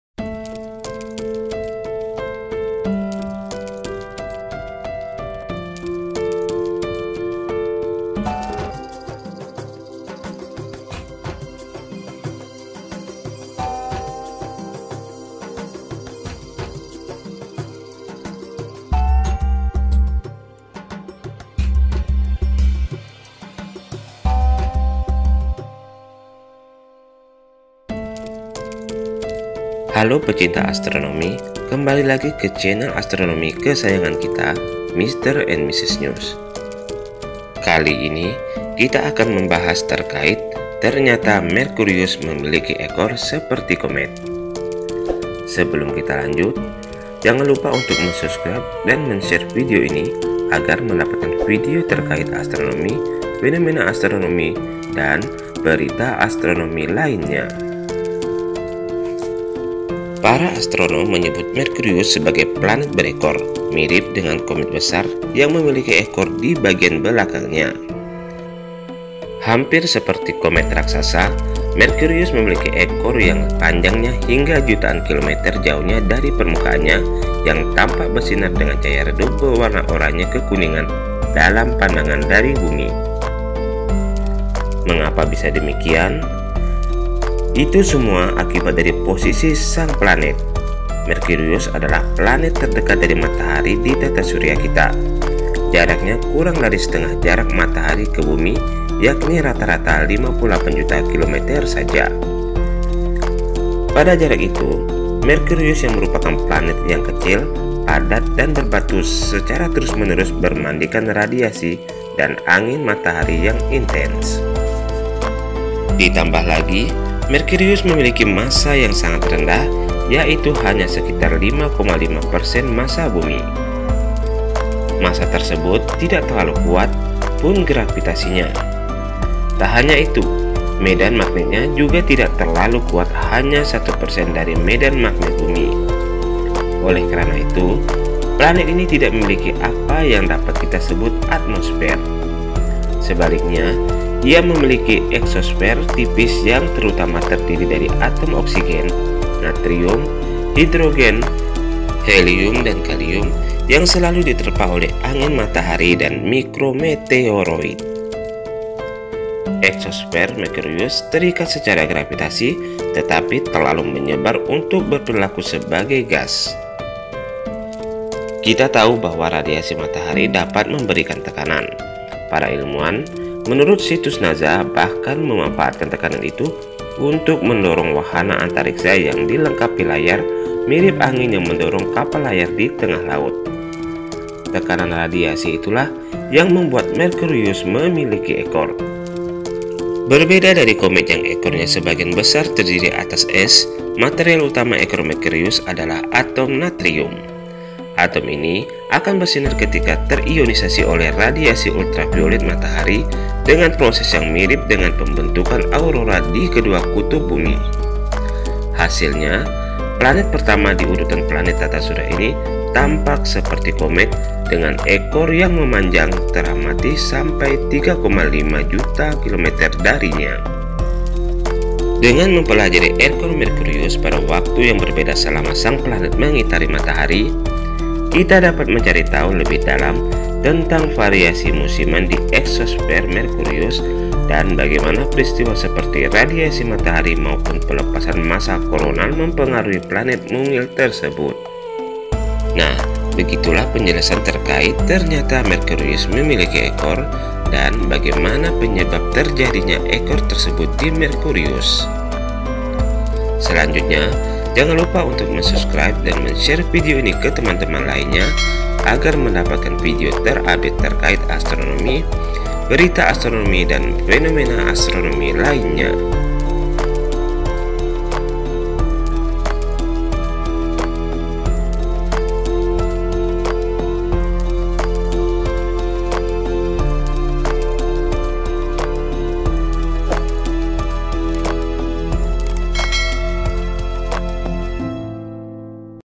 TERNYATA PLANET MERKURIUS MEMILIKI EKOR SEPERTI KOMET ( Mercury has a comet like tail )
TERIMKASIH !!

Para astronom menyebut Merkurius sebagai planet berekor, mirip dengan komet besar yang memiliki ekor di bagian belakangnya.Ekor Merkurius memanjang jutaan kilometer dan bersinar dengan cahaya redup berwarna kuning-oranye. Hal ini diakibatkan oleh posisi planet.Merkurius merupakan planet terdalam di tata surya. Jarak orbit Merkurius dengan Matahari rata-rata 58 juta kilometer. Dengan jarak sedekat itu membuat Merkurius selalu terpapar radiasi dan diterpa angin Matahari.Terlebih karena Merkurius memiliki massa yang sangat rendah, planet berbatu itu tidak terlalu kuat secara gravitasi. Medan magnetnya pun hanya 1 persen dari Bumi. Karena itu, Merkurius tidak memiliki atmosfer. Sebaliknya, planet tersebut memiliki eksosfer tipis yang terdiri dari atom oksigen, natrium, hidrogen, helium, dan kalium.
Eksosfer itu terikat secara gravitasi dengan planet, tetapi terlalu menyebar sebagai gas. Tekanan radiasi Matahari pada permukaan Merkurius menciptakan ekor, proses serupa yang juga terjadi pada komet.Perbedaannya, ekor komet dihasilkan dari es yang menyublim, sementara ekor Merkurius terbuat dari atom natrium. Bahan tersebut akan bersinar ketika terionisasi oleh radiasi ultraviolet Matahari. Hasilnya, Merkurius tampak seperti komet dengan ekor yang telah diamati mengalir hampir 3,5 juta kilometer dari planet tersebut.
para astronom mengatakan, ekor Merkurius memiliki keistimewaan karena dapat memberikan informasi tentang variasi musiman di eksosfer Merkurius dan bagaimana peristiwa seperti jilatan api Matahari serta pelepasan massa koronal memengaruhi planet.Selain itu, karena ekor natrium ditemukan terkait dengan planet berbatu seperti Merkurius, mengidentifikasi natrium dalam sistem di sekitar bintang lain dapat membantu para ahli melacak planet ekstrasurya berbatu dan menilai potensi kelayakhuniannya.